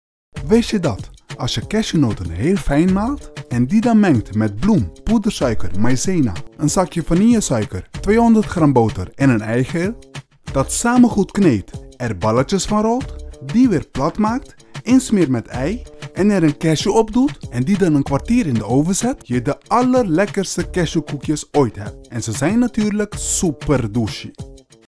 Cashewnoten koekjes - Recept: in de comments!
CASHEWKOEKJES • Go make 'em!
200 gr roomboter
100 gr poedersuiker
1 eidooier
1 zakje vanillesuiker
250 gr tarwebloem
60 gr maïzena
200 gr gemalen cashewnoten
En verder...
1 ei & extra cashewnoten
1️⃣ Meng alle ingrediënten uit de eerste lijst tot 'n deeg, rol er balletjes van en druk die plat op een bakplaat.
2️⃣ Verwarm de oven voor op 170°C.
3️⃣ Besmeer de koekjes met ei, duw er 'n cashewnoot in en bak ze 15 minuten.
Eet smakelijk / Kome Dushi!